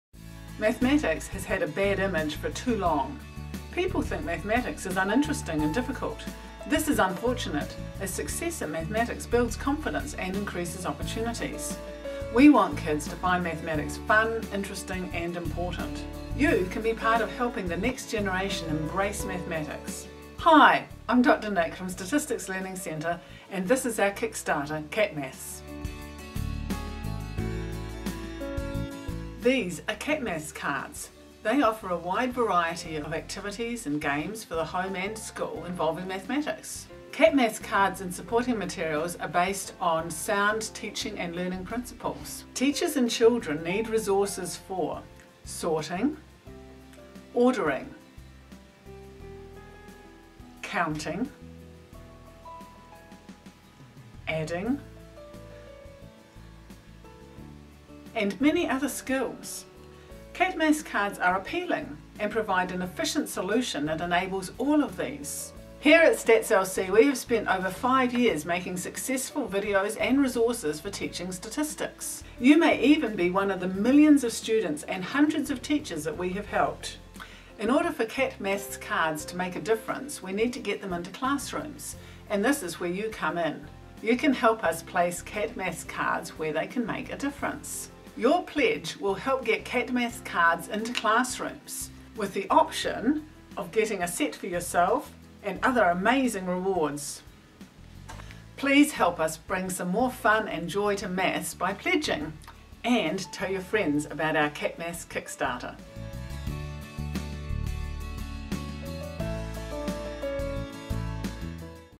Cat Maths - Kickstarter
In this video, Dr Nic introduces Cat Maths cards, and the Kickstarter campaign which will run from 12th October to 9 November 2016.